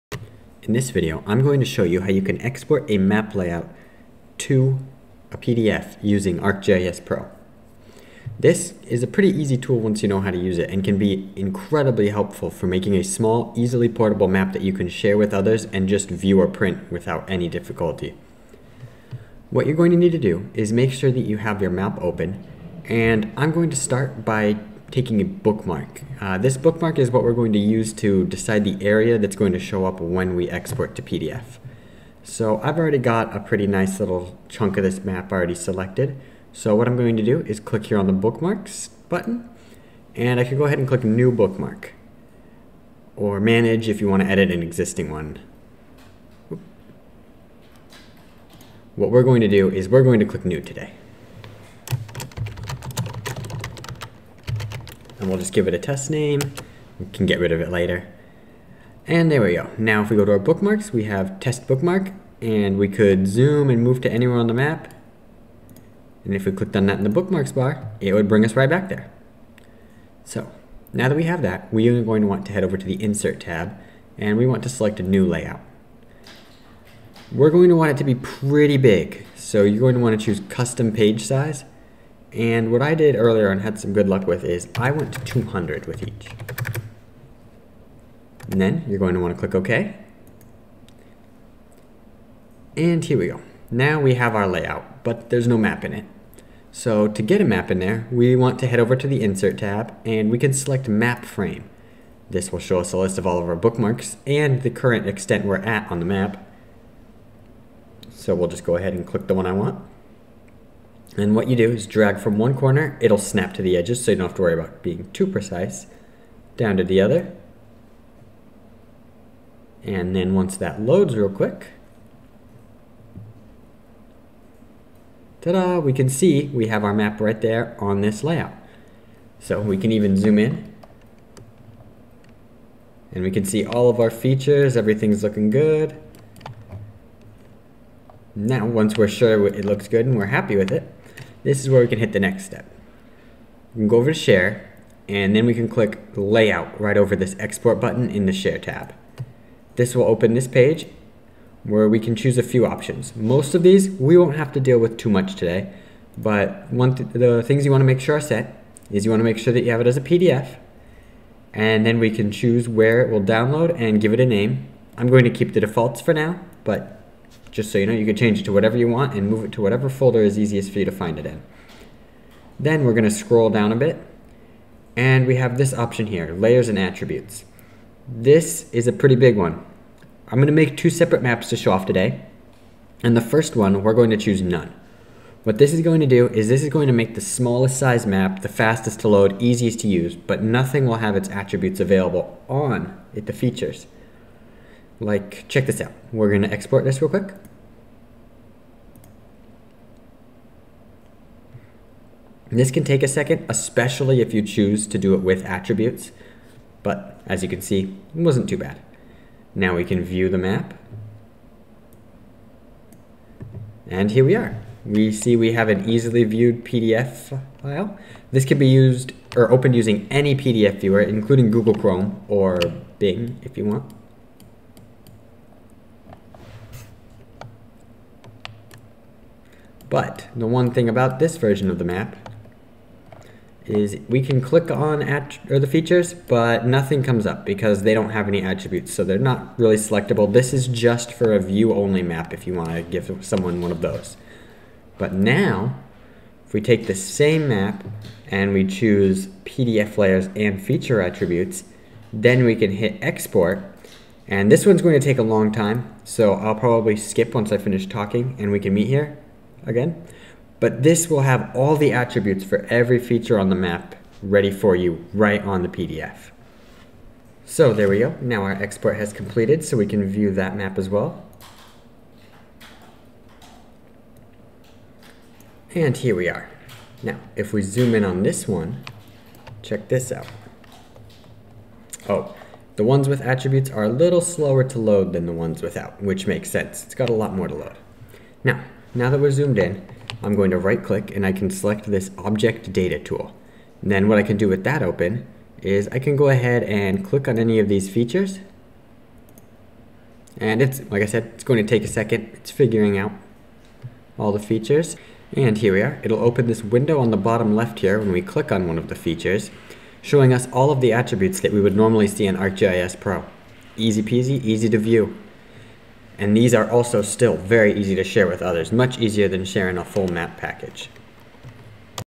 Exporting a Map Layout to PDF in ArcGIS Pro
Here in this video I show you how you export a layout to PDF format.

Learn more about how OpenPoint is helping utilities increase efficiency and improve customer service